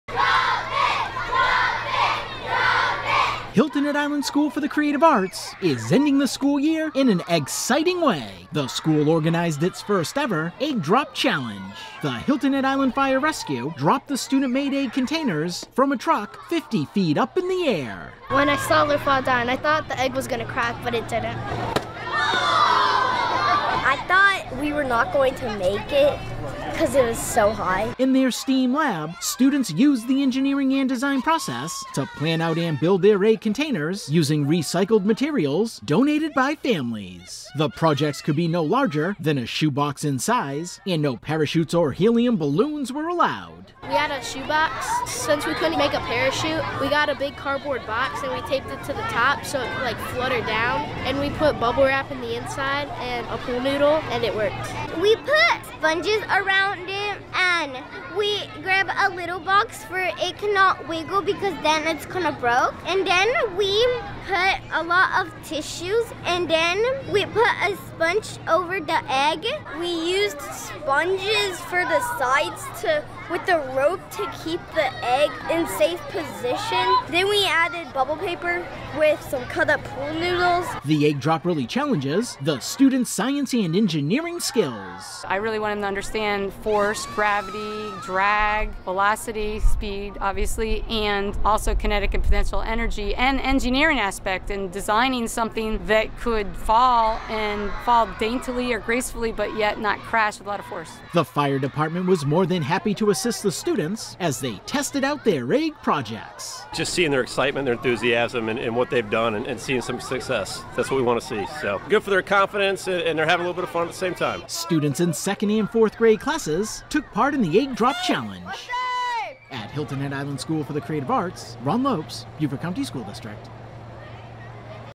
Egg-citing Egg Drop Challenge
Hilton Head Island School for the Creative Arts held an Egg Drop Challenge, dropping the egg projects off a fire truck from way up high.